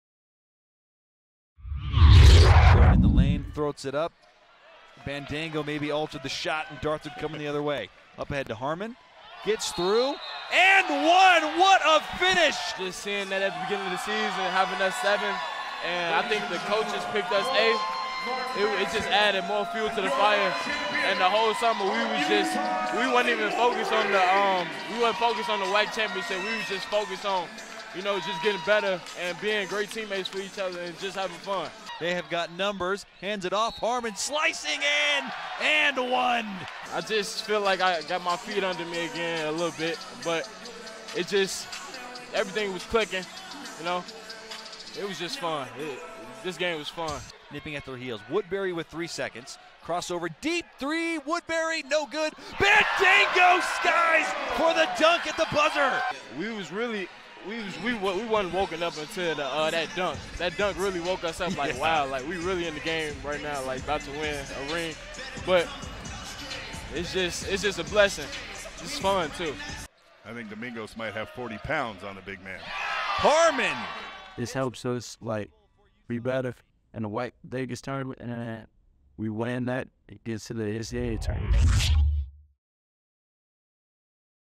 UVU Basketball: Wolverines Clinch Share of WAC Regular Season Title
Utah Valley University clinched a share of the Western Athletic Conference Regular season title after defeating UT Arlington at home. This is the second time in four years the the Wolverines has claimed a share of the title. Utah Valley can clinch an outright title on Friday which would be the programs second outright title in program history.